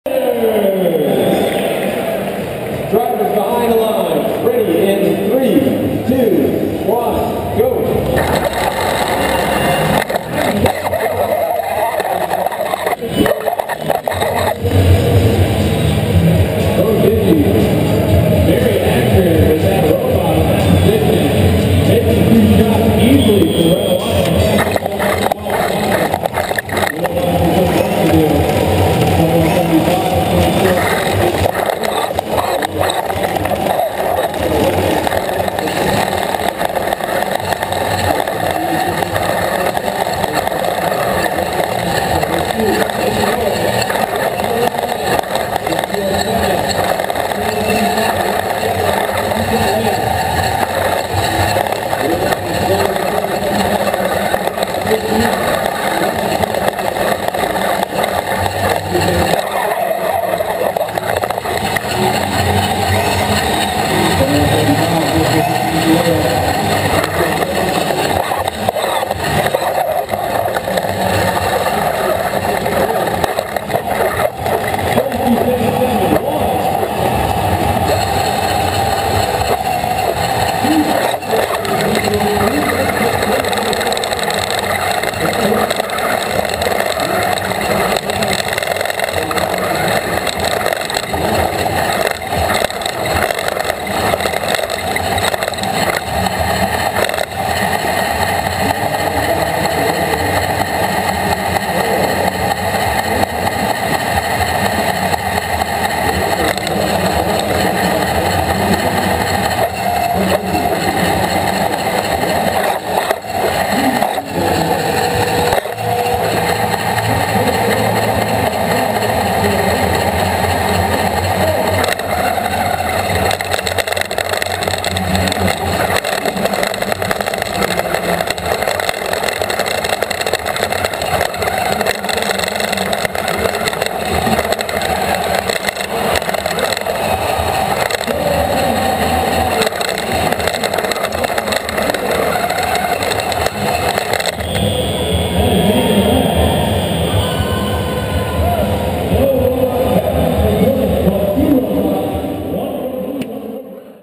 FRC Team 2207 Robot POV Saturday Match 2
We decided to mount my GoPro onto the robot vs. me carry it. So this is footage from that during our Saturady Match #1. This is straight video